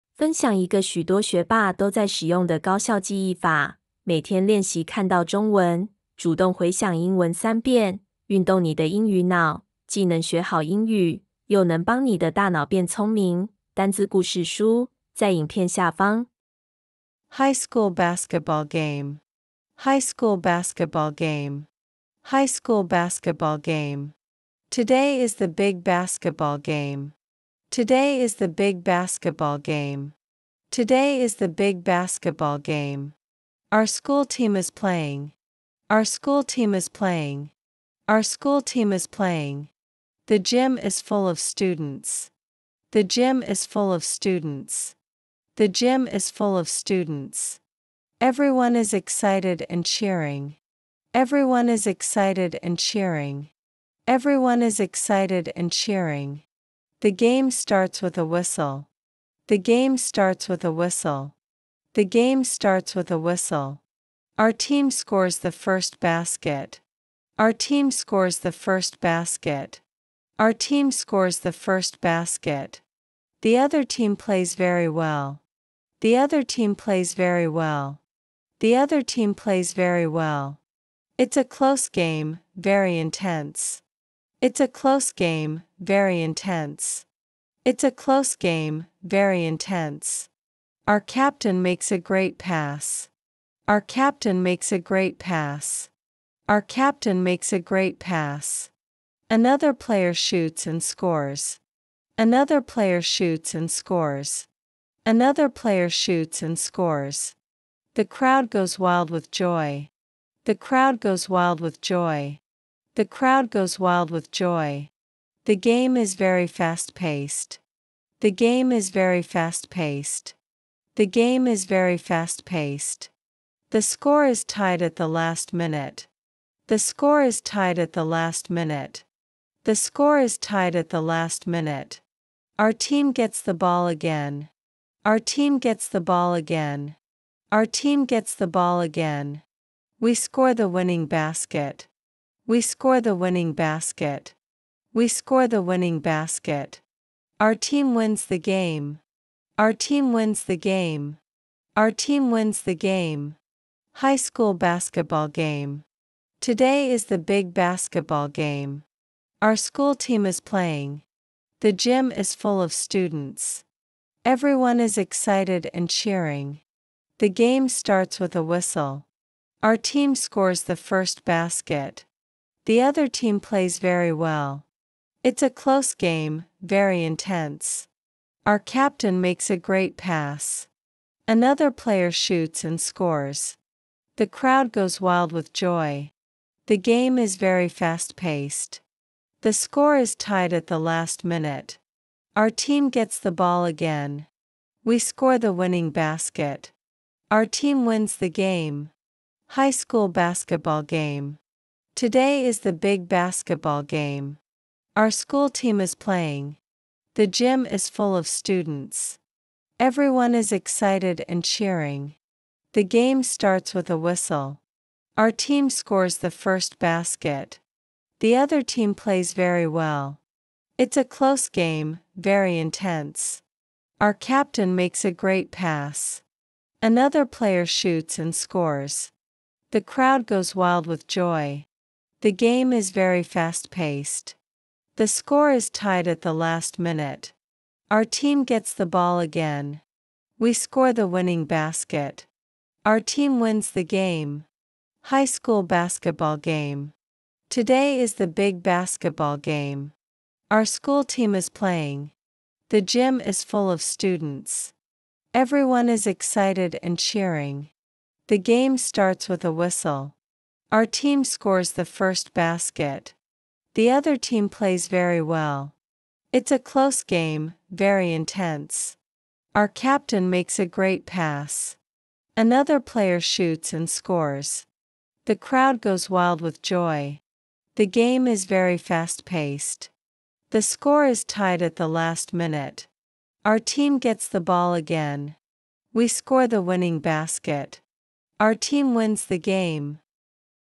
高中5000單字故事(一) 02 高中籃球賽 | 英語小天才 高效記憶法 | 學霸都在使用的高效記憶法 | 練習看到中文 主動回想英文 運動英語腦 | 國中英語2000單字故事書
英語小天才 高效記憶法 單字故事書
分享一個許多學霸都在使用的高效記憶法 :
每天練習看到中文，主動回想英文三遍，運動你的英語腦，
既能學好英語，又能鍛鍊大腦，歡迎免費下載